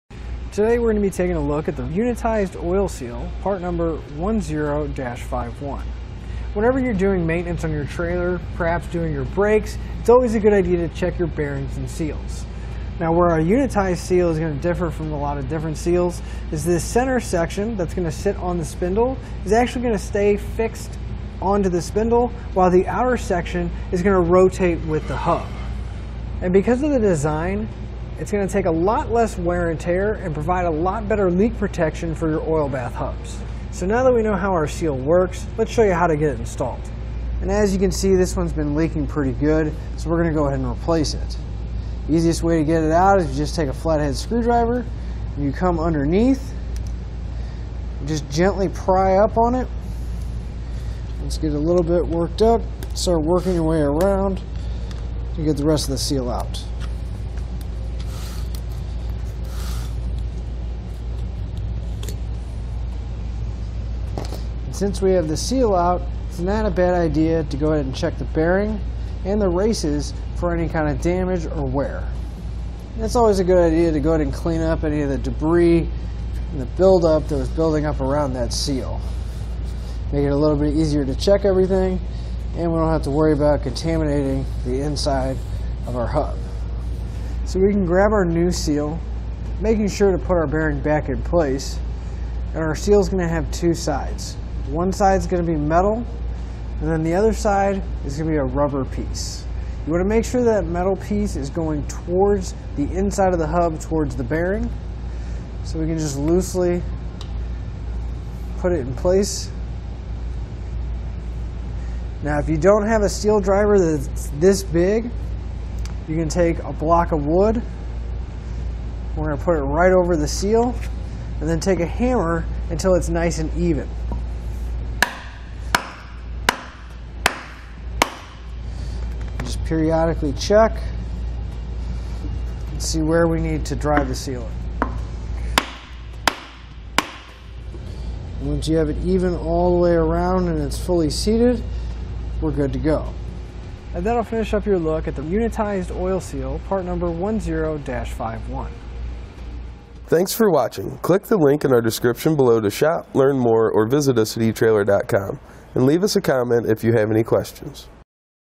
etrailer | Unitized Oil Seal Review
Click for more info and reviews of this Redline Trailer Bearings Races Seals Caps:

Speaker 1:          Today we're going to be taking a look at the Unitized Oil Seal, part number 10-51. Whenever you're doing maintenance on your trailer, perhaps doing your brakes, it's always a good idea to check your bearings and seals. Now where our Unitized seal is going to differ from a lot of different seals is this center section that's going to sit on the spindle, is actually going to stay fixed onto the spindle while the outer section is going to rotate with the hub.And because of the design, it's going to take a lot less wear and tear and provide a lot better leak protection for your oil bath hubs. So now that we know how our seal works, let's show you how to get it installed. And as you can see this one's been leaking pretty good, so we're going to go ahead and replace it.Easiest way to get it out is just take flathead screwdriver, and you come underneath, and just gently pry up on it. And just get it a little bit worked up, so working your way around, and you get the rest of the seal out.

Since we have the seal out, it's not a bad idea to go ahead and check the bearing in the races for any kind of damage or wear.It's always a good idea to go ahead and clean up any of the debris, and the buildup to building up around that seal. It'll make it a little bit easier to check everything, and we don't have to worry about contaminating the inside of our hub.So we can grab our new seal, making sure to put our bearing back in place, and our seal is going to have two sides. One side's going to be metal, and then the other side is going to be a rubber piece. You want to make sure that metal piece is going towards the inside of the hub towards the bearing, so we can just loosely put it in place.Now if you don't have a seal driver that is this big, you can take a block of wood. We're going to put it right over the seal, and then take a hammer until it's nice and even.

Just periodically check, and see where we need to drive the seal. Once you have it even all the way around and it's fully seated, we're good to go.And that'll finish up your look at the Unitized Oil Seal, part number 10-51.